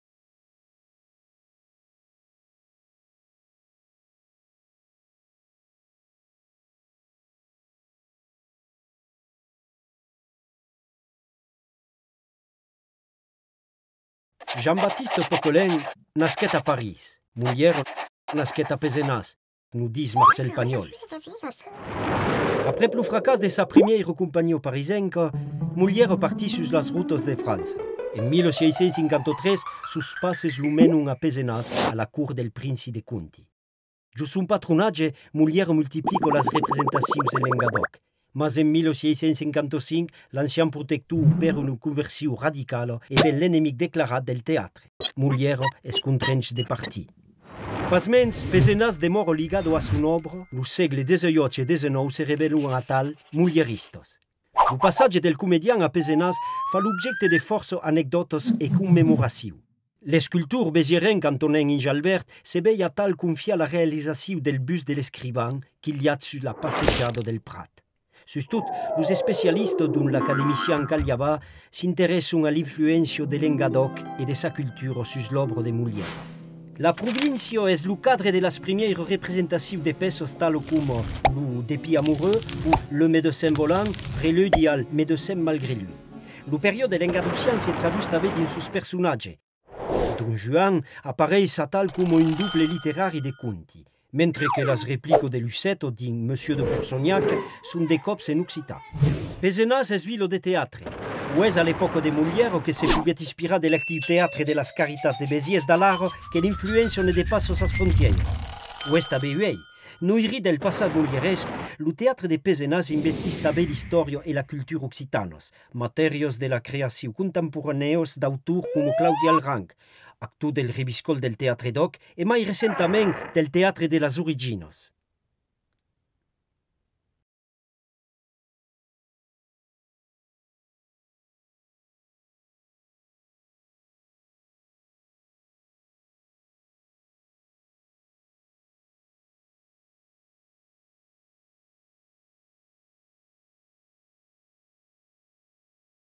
Moliere e lo teatre d'òc VOSTFR - Occitanica eAnem / CIRDOC
Molière et le théâtre d'oc
Videoguidas de veire sus la mediatèca numerica collaborativa occitana
Version occitane sous-titrée en français